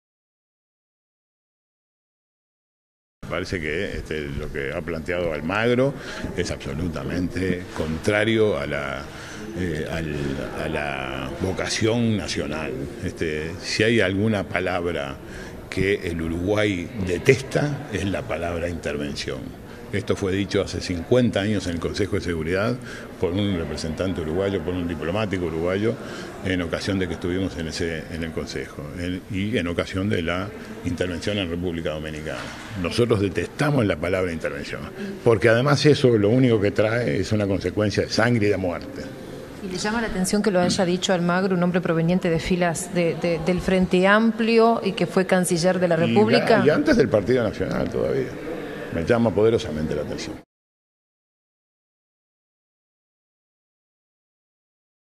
Gobierno rechazó dichos de Almagro sobre intervención militar en Venezuela
El canciller Nin Novoa dijo que las declaraciones del secretario General de la OEA le llaman “poderosamente la atención”.